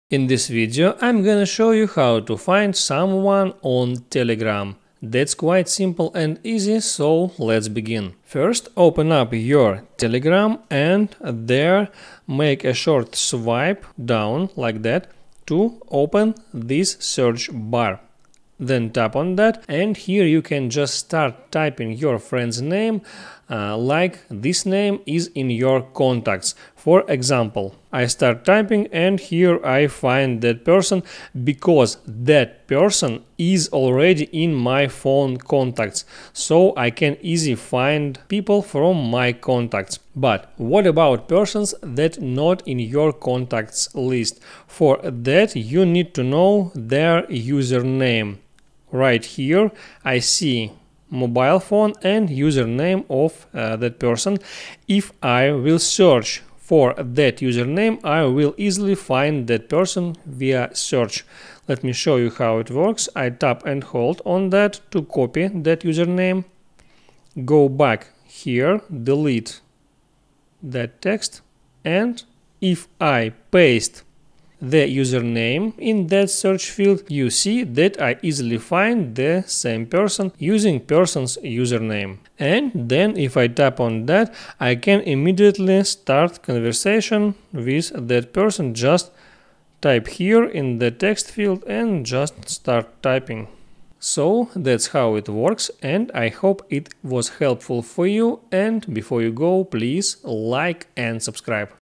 How To Find A User on Telegram. [Find Your Friend on Telegram]
In this video i show simple way How To Find A User on Telegram